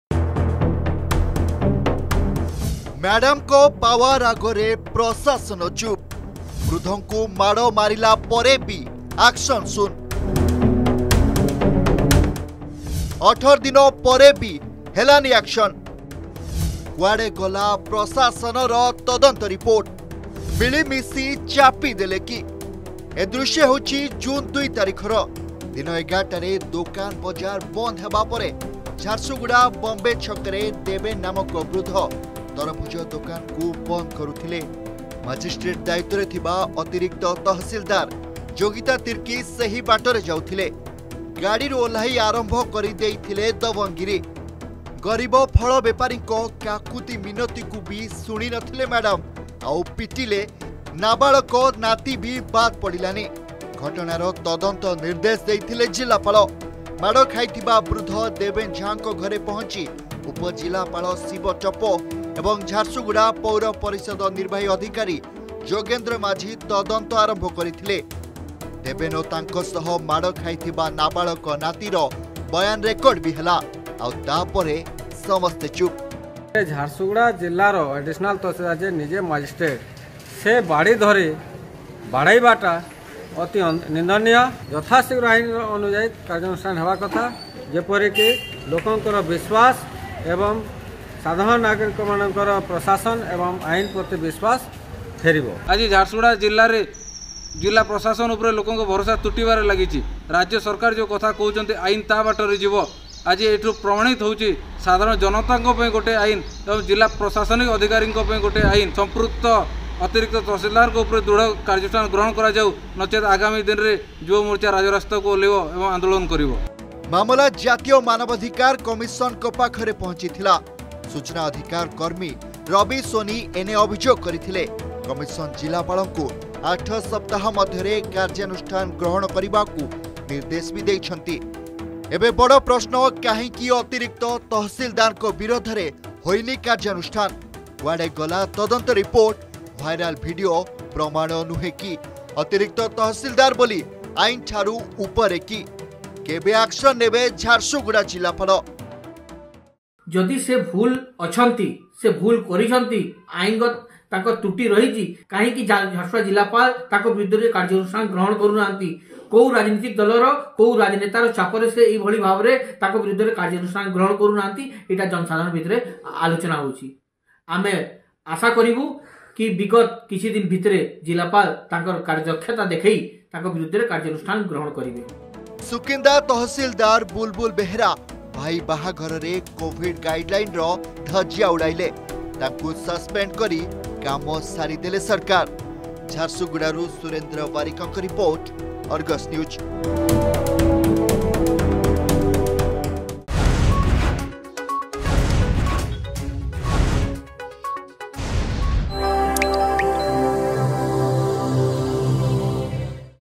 ମ୍ୟାଡମଙ୍କ ପାୱାର ଆଗରେ ପ୍ରଶାସନ ଚୁପ୍
Jharsuguda: No Action Taken Against Lady Tehsildar Who Had Beaten Up An Aged Street Vendor & Minor Grandson.
ମ୍ୟାଡମଙ୍କ ପାୱାର ଆଗରେ ପ୍ରଶାସନ ଚୁପ୍  I 18 ଦିନ ପରେ ବି ହେଲାନାହିଁ ଆକସନ୍ I କୁଆଡେ ଗଲା ପ୍ରଶାସନର ତଦନ୍ତ ରିପୋର୍ଟ I ସରକାରୀ ଅଫିସର ବୋଲି  ମିଳିମିଶି ଚାପିଦେଲେ କି I